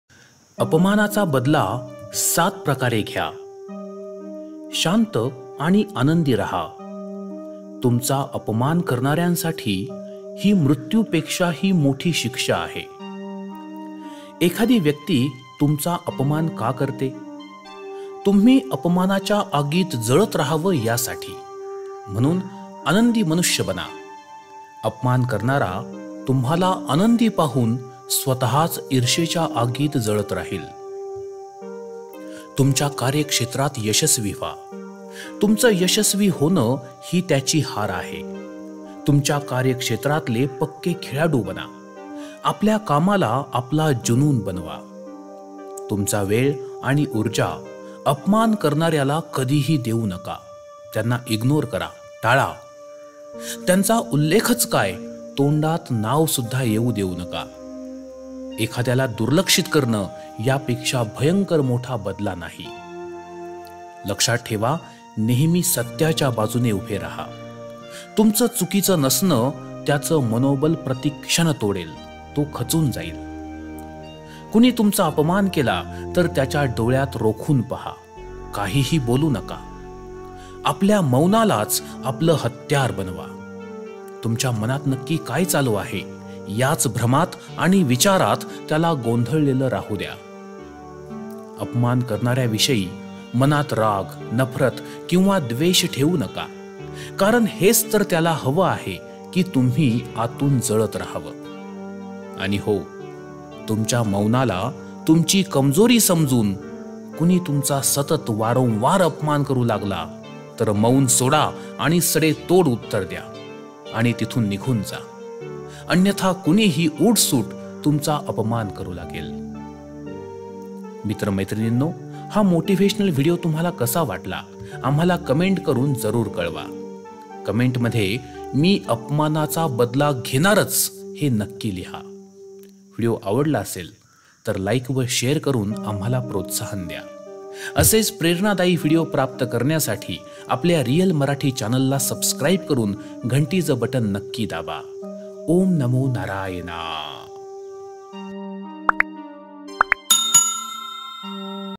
अपमानाचा बदला.. ७ प्रकारे घ्या || How to Avenge an Insult.. 7 Powerful ways (in Marathi)
This Marathi motivational video will teach you How to Avenge an Insult in 7 powerful ways.
जर कोणी सारखा सारखा अपमान करत असेल तर त्या अपमानाचा बदला ७ प्रकारे कसा घ्यावा हे सांगणारा हा प्रेरणादायी व्हिडिओ आहे.

अपमानाचा बदला.. ७ प्रकारे घ्या

१. शांत आणि आनंदी राहा.. तुमचा अपमान करणाऱ्यांसाठी ही मृत्यूपेक्षाही मोठी शिक्षा आहे..

२. एखादी व्यक्ति तुमचा अपमान का करते? तुम्ही अपमानाच्या आगीत जळत राहावं यासाठी.. म्हणून आनंदी मनुष्य बना! अपमान करणारा तुम्हाला आनंदी पाहून स्वतःच ईर्षेच्या आगीत जळत राहील.

३. तुमच्या कार्यक्षेत्रात यशस्वी व्हा! तुमचे यशस्वी होणे, ही त्याची हार आहे.. तुमच्या कार्यक्षेत्रातले पक्के खेळाडू बना. आपल्या कामाला आपला जुनुन बनवा.

४. तुमचा वेळ आणि ऊर्जा अपमान करणाऱ्याला कधीही देऊ नका. त्यांना ignor करा.. टाळा.. त्यांचा उल्लेखच काय, तोंडात नाव सुद्धा येऊ देऊ नका. एखाद्याला दुर्लक्षित करणे, यापेक्षा भयंकर मोठा बदला नाही.

५. लक्षात ठेवा, नेहमी सत्याच्या बाजूने उभे रहा. तुमचे चुकीचे नसणे, त्याचे मनोबल प्रतिक्षण तोडेल. तो खचून जाईल.

६. कुणी तुमचा अपमान केला, तर त्याच्या डोळ्यांत रोखून पहा, काहीही बोलू नका. आपल्या मौनालाच आपले हत्यार बनवा. तुमच्या मनात नक्की काय चालू आहे, याच भ्रमात आणि विचारात त्याला गोंधळलेले राहू द्या.

७. अपमान करणाऱ्या विषयी मनात राग, नफरत किंवा द्वेष ठेऊ नका. कारण हेच तर त्याला हवे आहे की तुम्ही आतून जळत राहावं.

७.१. आणि हो! तुमच्या मौनाला तुमची कमजोरी समजून, कुणी तुमचा सतत, वारंवार अपमान करू लागला, तर मौन सोडा आणि सडेतोड उत्तर द्या...आणि तिथून निघून जा. अन्यथा कुणीही उठसूट तुमचा अपमान करू लागेल.

मित्र-मैत्रिनींनो, हा motivational व्हिडिओ तुम्हाला कसा वाटला, आम्हाला कमेन्ट करून जरूर कळवा. कमेन्ट मध्ये “मी अपमानाचा बदला घेणारचं” नक्की लिहा.

व्हिडिओ आवडला असेल, तर like व share करून आम्हाला प्रोत्साहन द्या.

ॐ नमो नारायणा